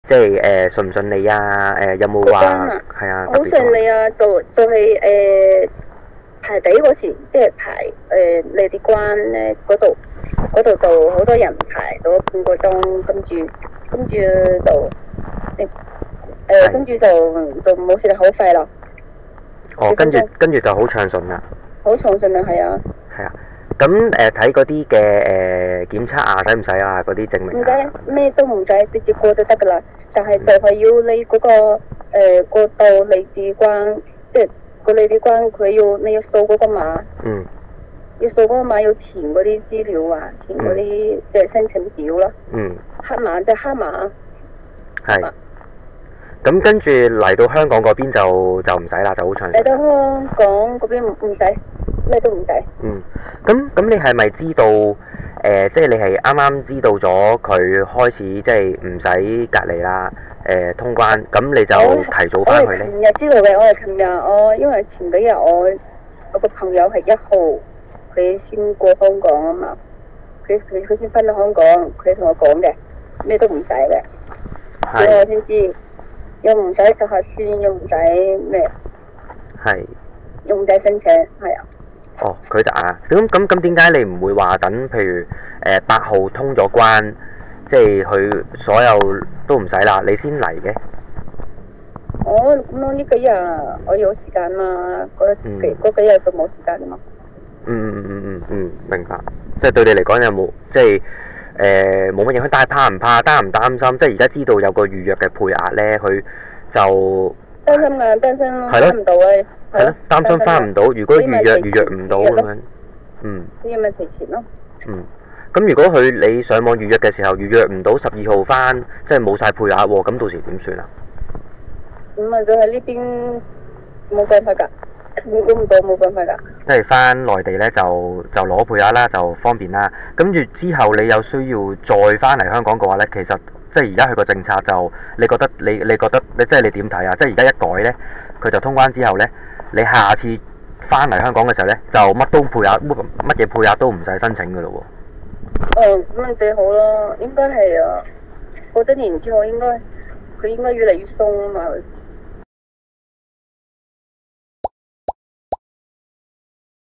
居住於深圳的香港市民Jenny 提早日程抵港探望親友及處理
居住於深圳的香港市民Jenny，日前得知1月8日通關的消息，即提早日程於今日（5日）抵港探望親友及處理要事，預計在通關日後再返回內地，屆時便毋須隔離。她說：「真係好開心通返關！無限住幾時去幾時返，對我呢種成日要兩邊走嘅人嚟講，真係好重要、好方便㗎！好似返返去疫情之前咁。」